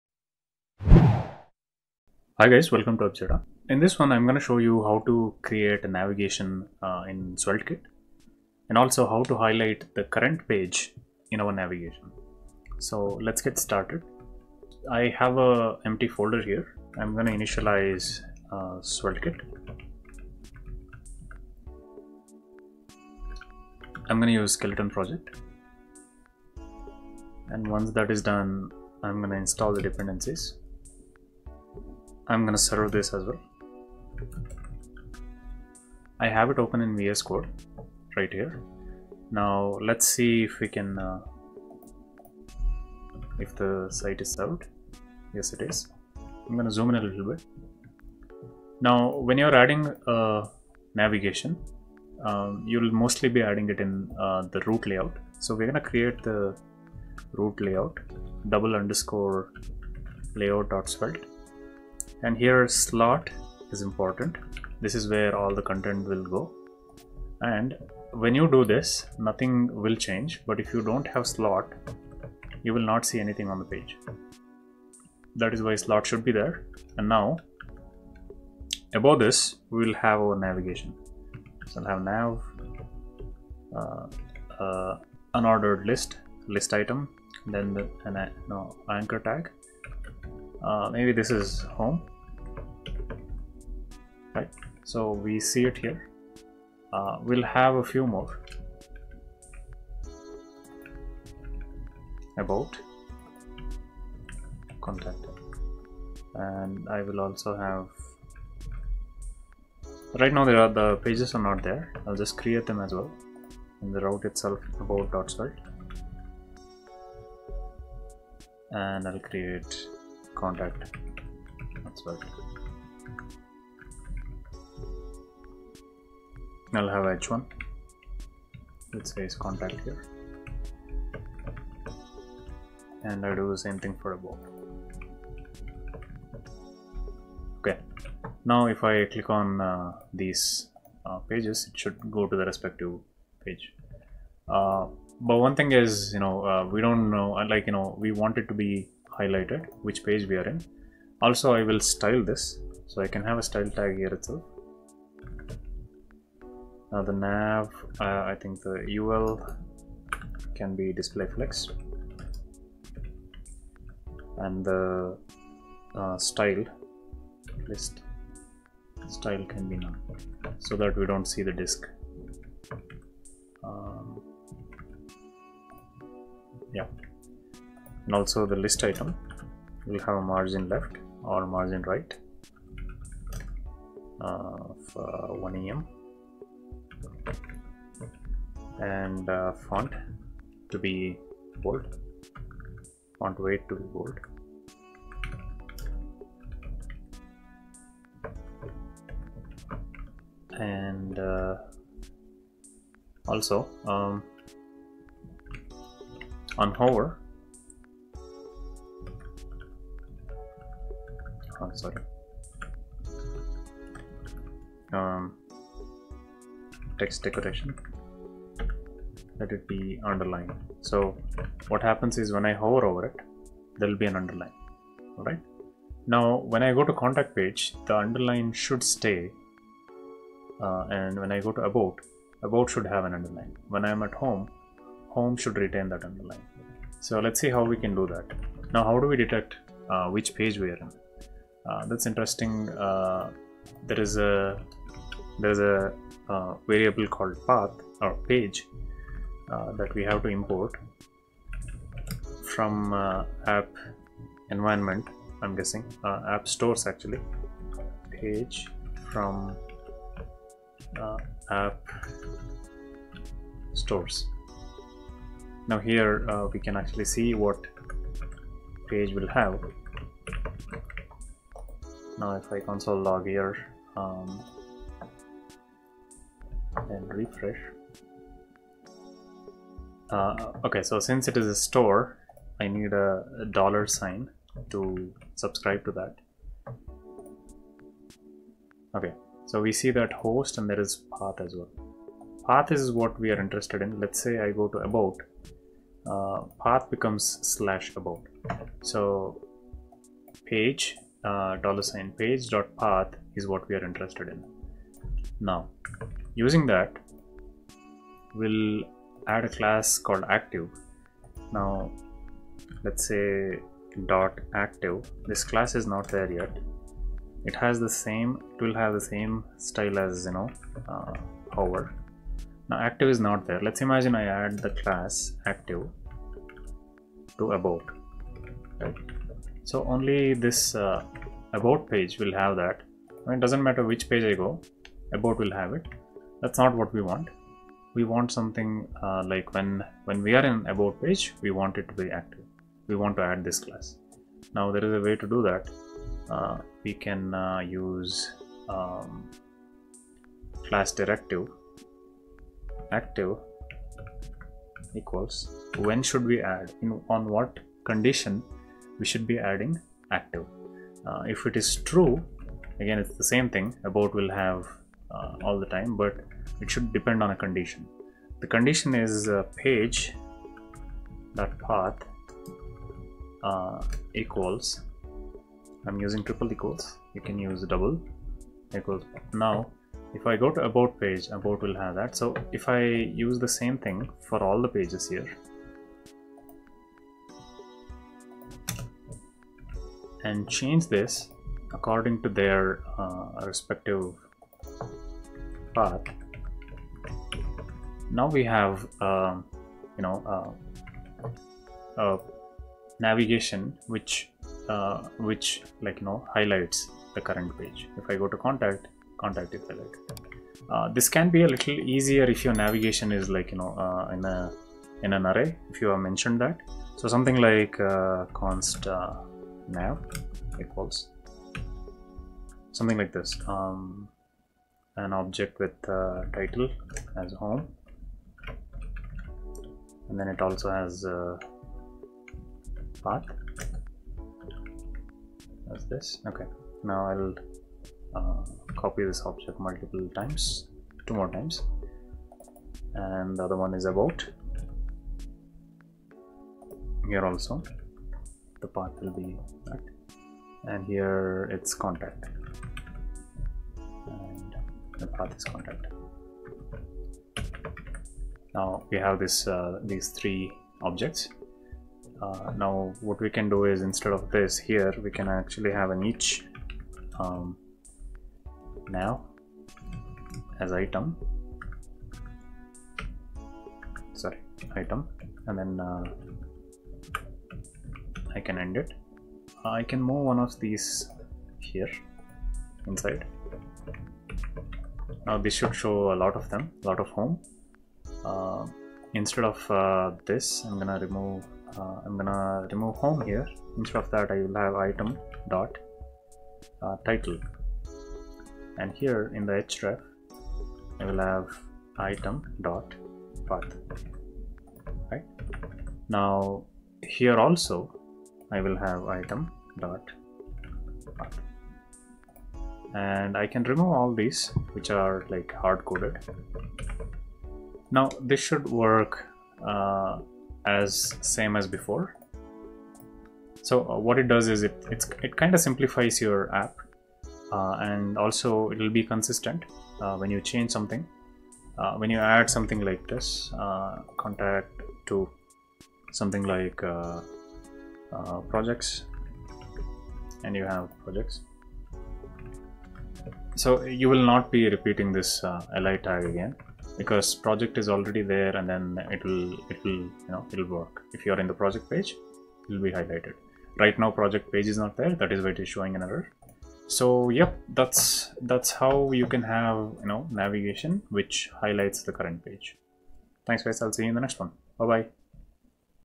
Sveltekit Navigation with Page Highlight
In this video, I will be showing you how to add a navigation or menu bar in Sveltekit website with current page highlight or active page highlight.

00:00 Intro
00:14 Sveltekit installation
00:48 Adding a Nav/Menu in Sveltekit
03:56 Highlight current page in Sveltekit
06:50 Simplify Sveltekit Navigation